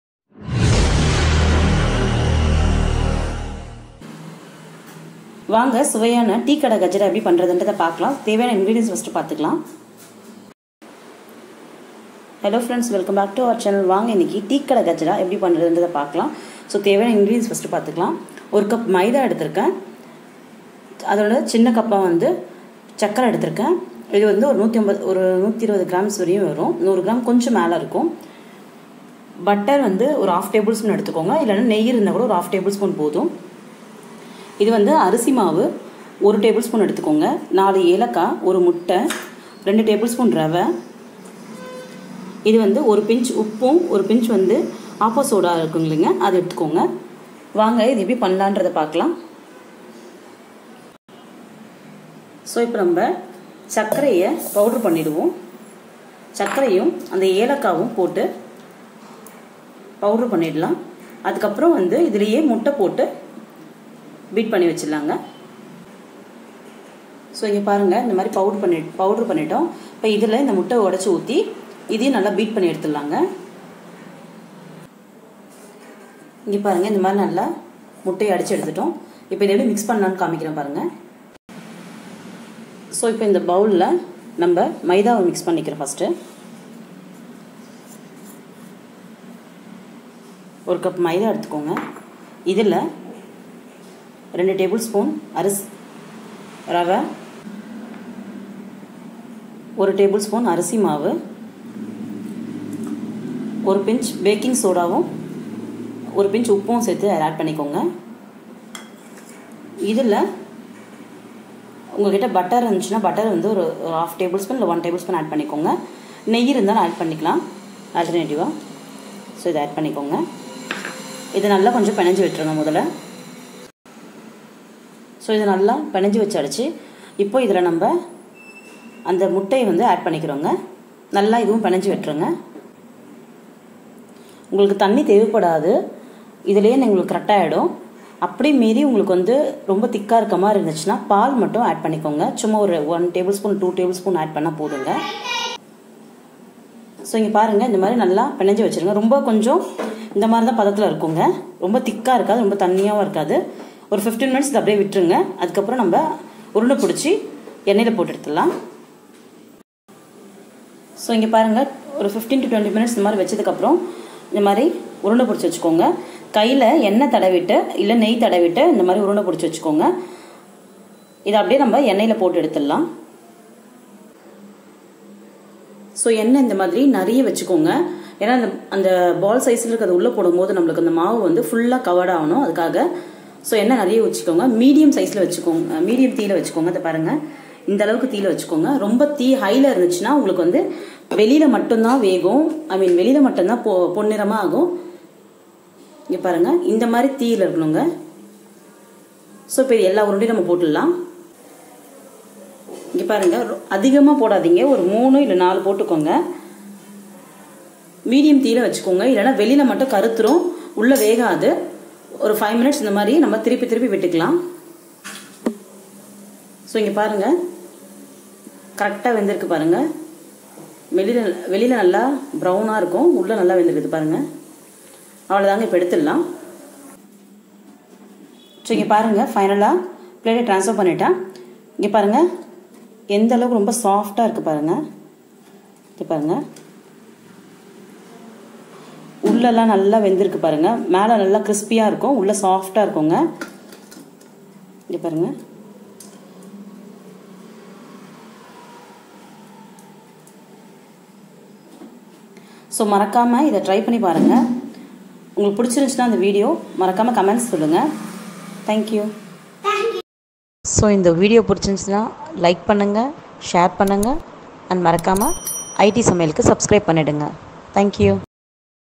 Tea shop kajada recipe in Tamil/டீ கடை கஜடா/tea shop vettu cake/ tea shop Vedi cake recipe in Tamil
How to make tea shop kajada recipe in Tamil/ Easy and tasty tea shop kajada recipe at home/ how to make vettu cake recipe in Tamil/ how to make Vedi cake recipe at home